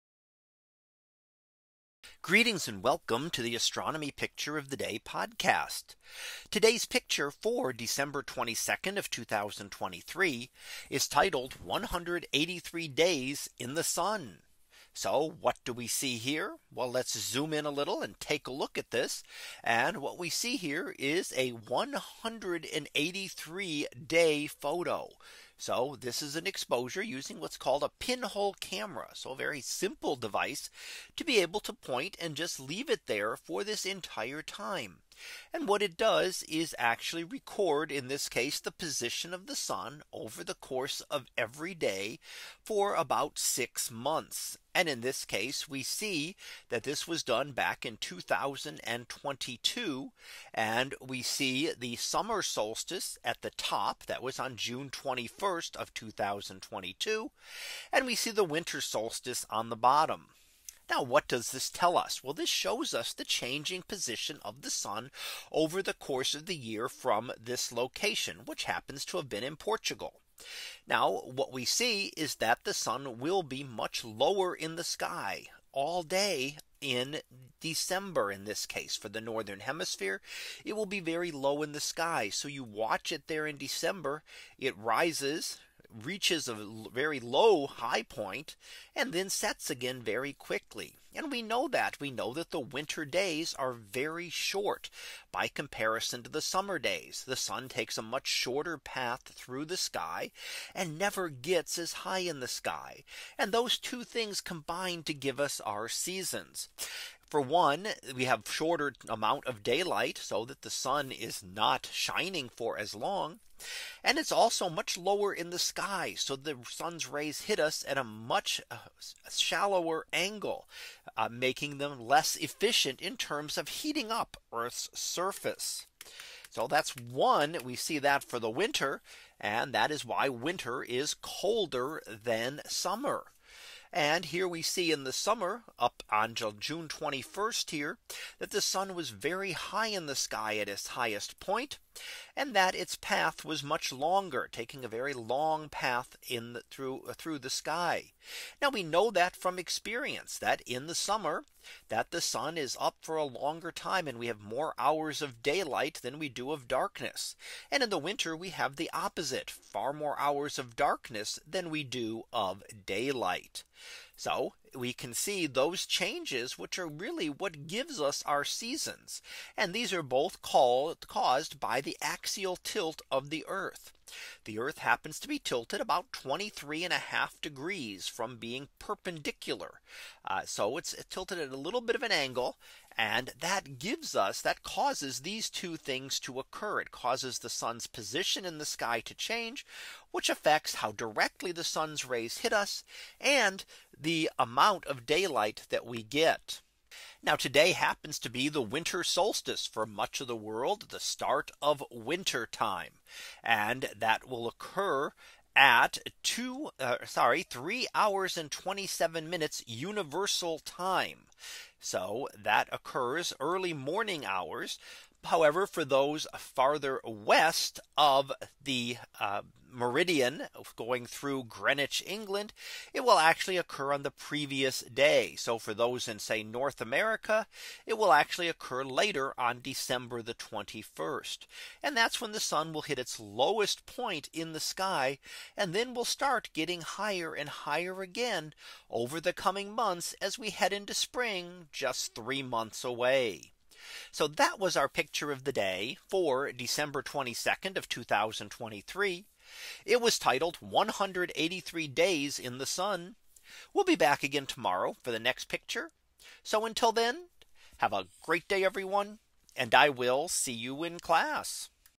2023 December 22 - 183 Days in the Sun
In today's image, we see a 183 day exposure of the Sun using a pinhole camera. This shows the position of the Sun over the course of the 6 month period from June to December of 2022.